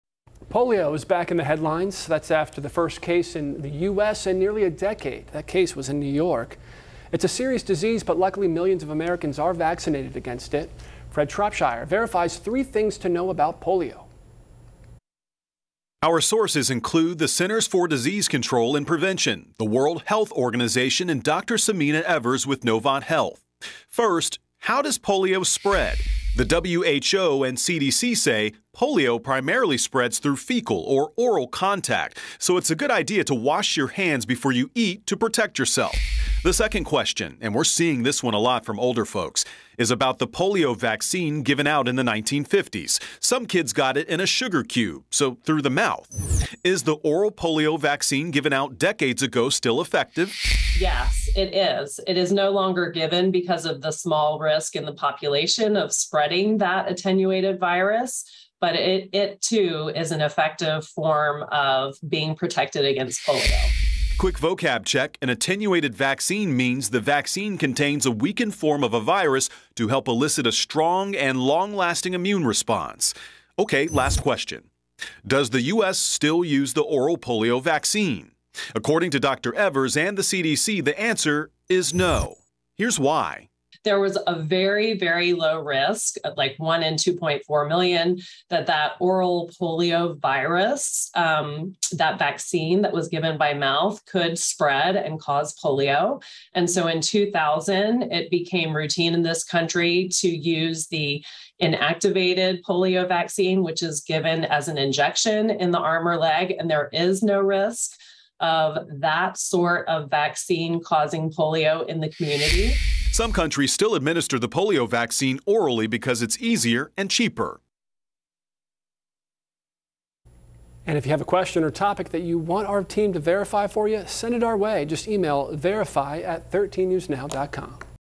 VERIFY: What you need to know about polio disease
Polio is back in the headlines after the first case in the U.S. in nearly a decade. It's a serious disease but luckily, millions of Americans are vaccinated against it.